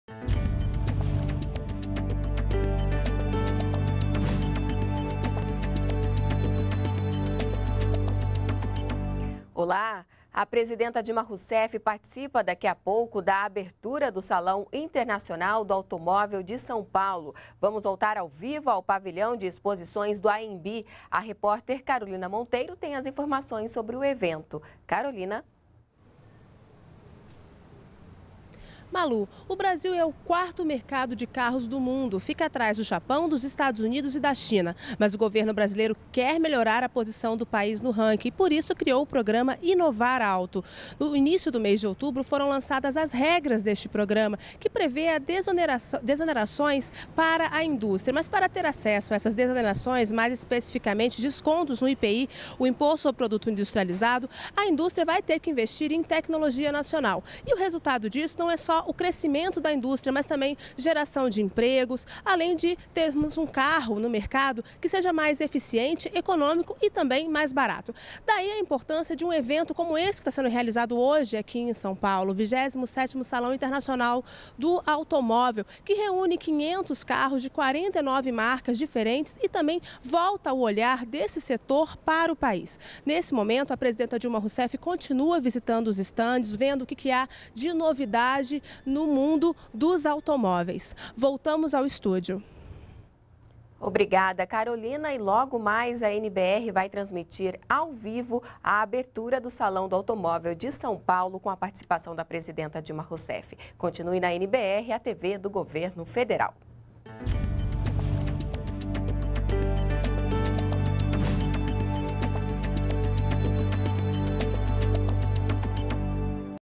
Governo quer subir posição do Brasil no ranking mundial do mercado de automóveis
ATIVIDADES DA PRESIDENTA - 24.10.12: A presidenta Dilma Rousseff está em São Paulo (SP), onde participa, nesta quarta-feira (24), da abertura do 27º Salão Internacional do Automóvel de São Paulo. O Brasil ocupa o quarto lugar no mercado de automóveis do mundo, ficando atrás do Japão, Estados Unidos e China. No início do mês de outubro foram lançadas as novas regras do programa Inovar Auto, criado pelo governo para aumentar a produção de automóveis e melhorar a posição do Brasil no ranking.